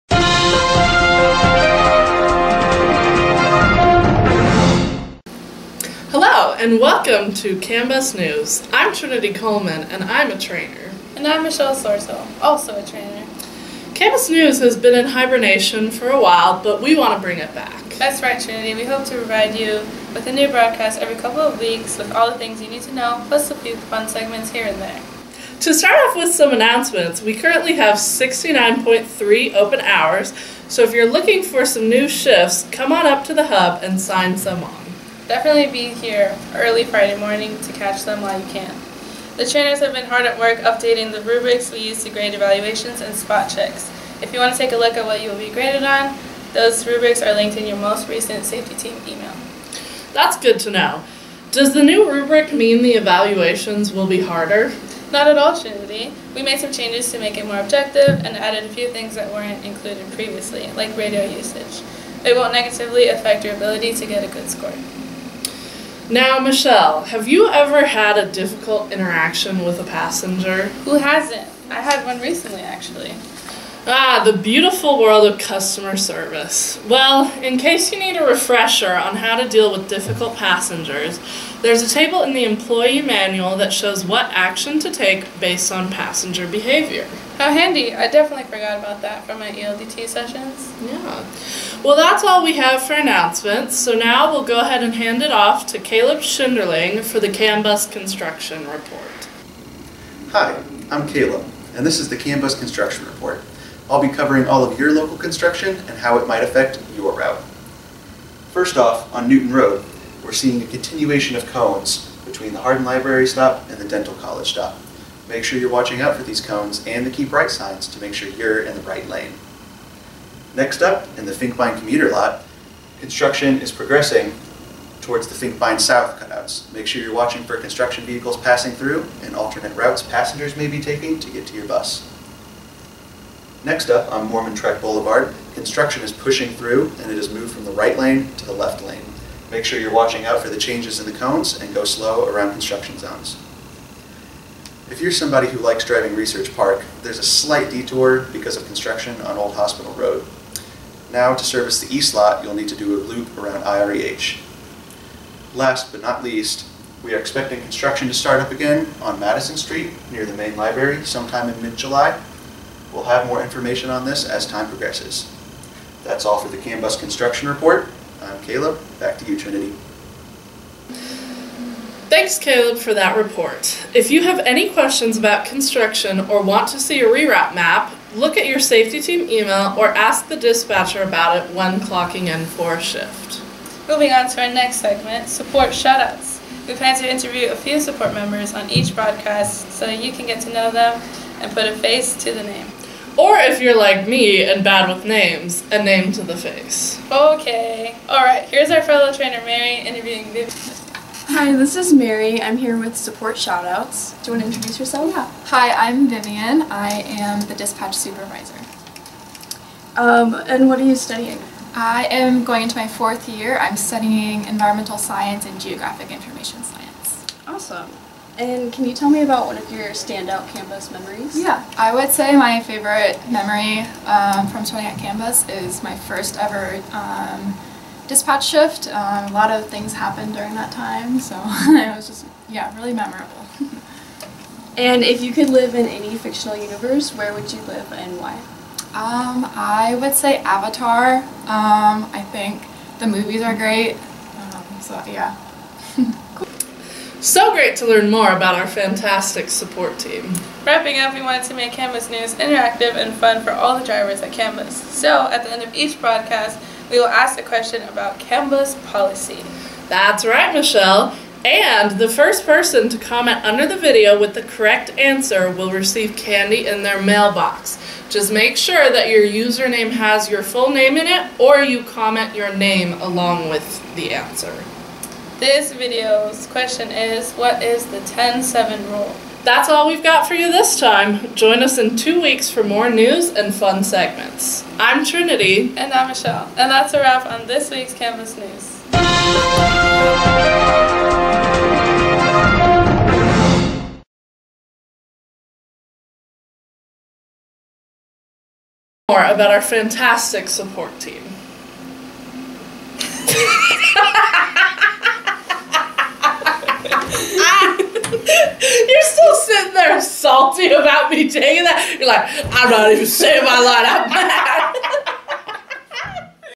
CAMBUS News 7.8.2024
CAMBUS News returns again! This week we bring you announcements, a construction report, and a support shout out! Stick around until the end for a CAMBUS trivia question and comment the answer for a piece of candy in your mailbox (01s only!)!!

Cast:
News Anchors: Trinity Coleman, Transit Operator Trainer and Michelle Sorto, Transit Operator Trainer
Construction Reporter: Caleb Schinderling, Transit Operator Trainer
Support Shoutout Interviewer: Mary Waite, Transit Operator Trainer
Support Shoutout Guest: Vivian Slack, Dispatch Supervisor

News Footage and Script by Mary Waite, Transit Operator Trainer
CAMBUS News produced and edited by Mary Waite, Transit Operator Trainer